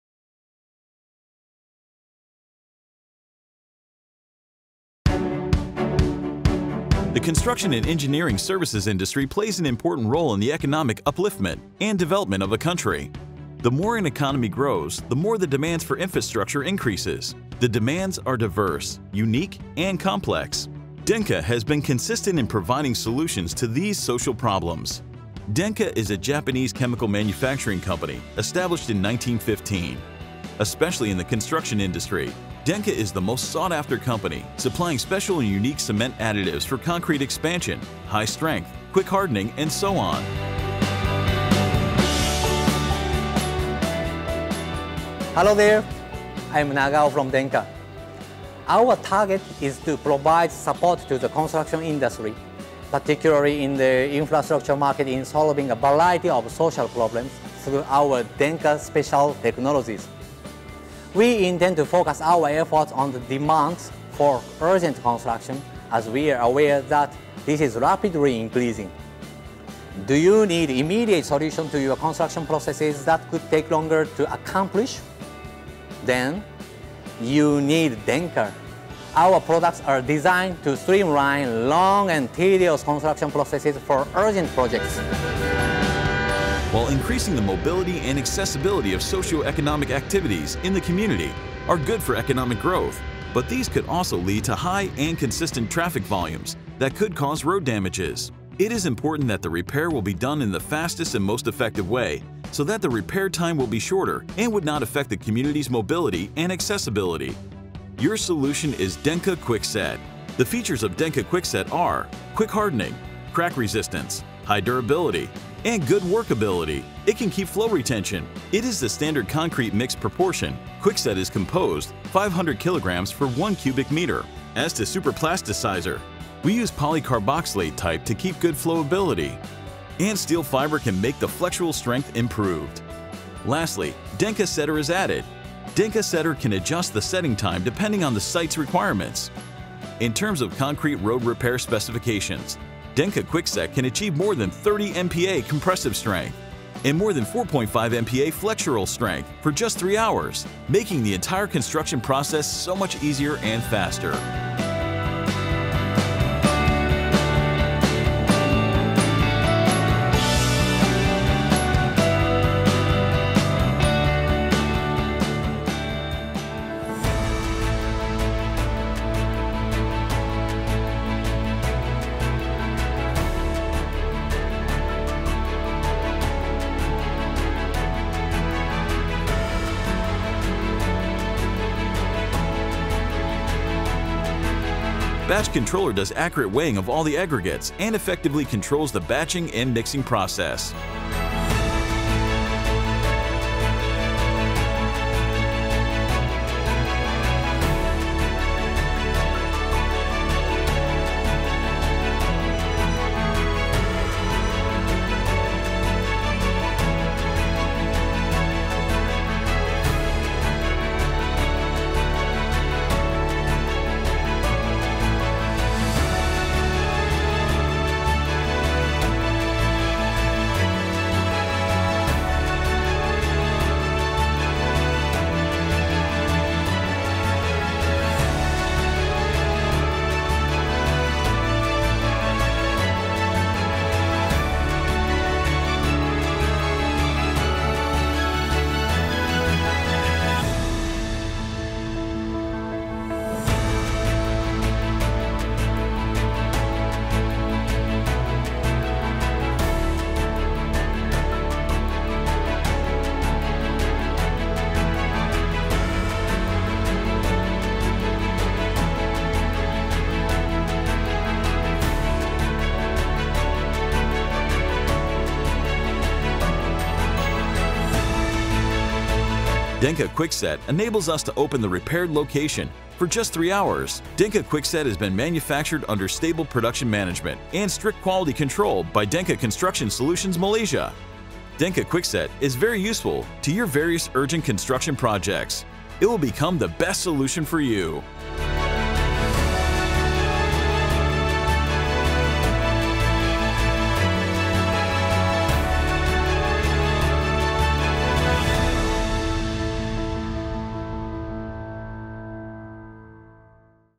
Denka Quickset - Ultra Quick Hardening Concrete for Emergency Road Repair
The features of Denka Quickset are Quick Hardening, Crack Resistance, High Durability, and Good workability.
It can keep flow retention.

In terms of concrete road repair specifications, Denka quickset can achieve more than 30 MPa compressive strength and more than 4.5 MPa flexural strength for just 3 hours, making the entire construction process so much easier and faster.